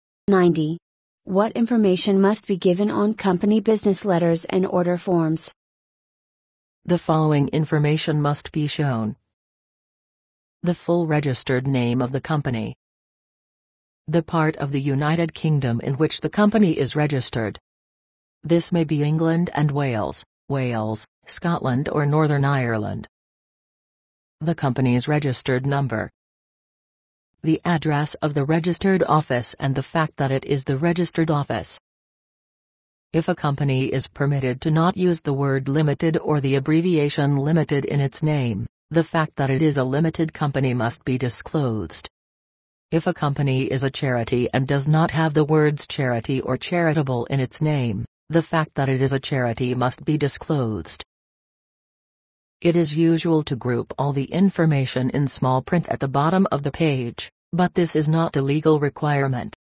What information must be given on company business letters and order forms?
The following information must be shown: ...